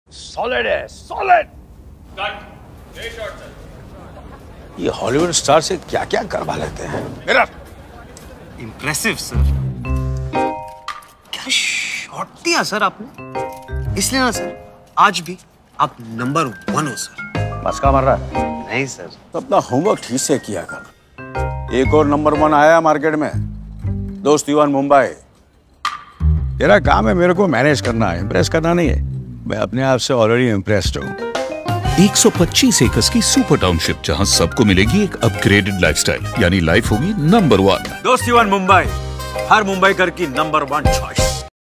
Launching Mumbai's premier housing development sprawled across 125 acres
Launching Mumbai's premier housing development sprawled across 125 acres. Indulge in the comfort of well-planned homes within a gated township. Live the No.1 life at Dosti 1 Mumbai with 12 unique brand alliances & 40+ amenities for Lifestyle, Education, Sports, Spirituality, Culture & Healthcare.

or BKC Experience Center for a first-of-it's-kind home buying experience.

1 Bed Homes Starting 39.99 Lakh All Inclusive. 2 Bed Homes Starting 55.99 Lakh All Inclusive.

PAY JUST 10% NOW & NOTHING TILL POSSESSION*

Project RERA Registered